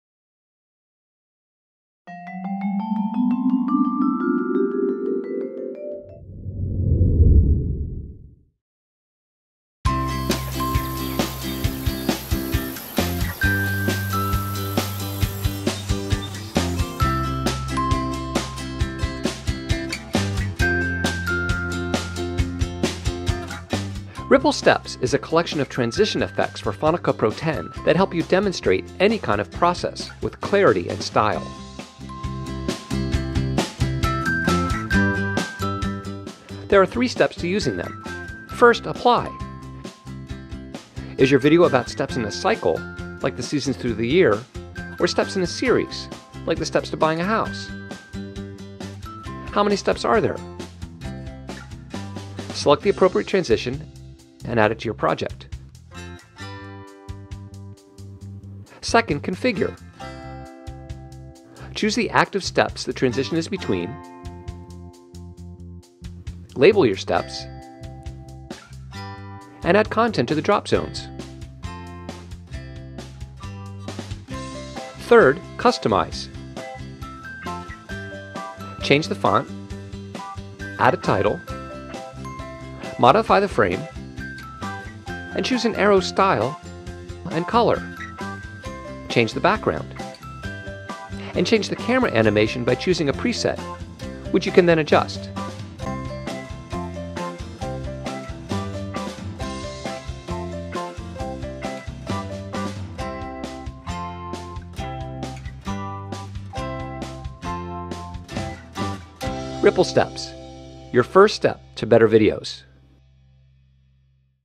Ripple Steps for Final Cut Pro X
Transition effect plugins to graphically demonstrate a procedure or process in FCP X.